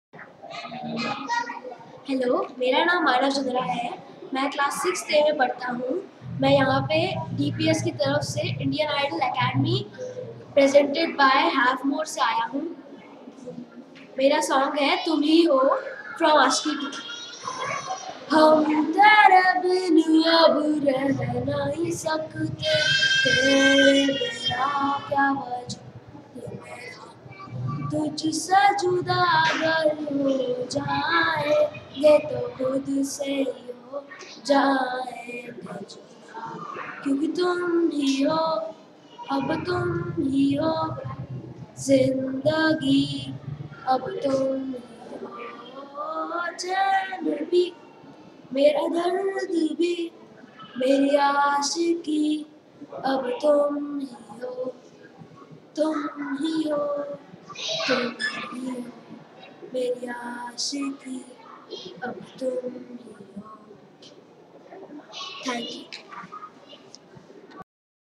MAnav Chandra - DPS School (6th Standard)
MAnav Chandra - DPS School (6th Standard) is singing a song for Indian Idol Academy Festival Of Music presented by Havmor.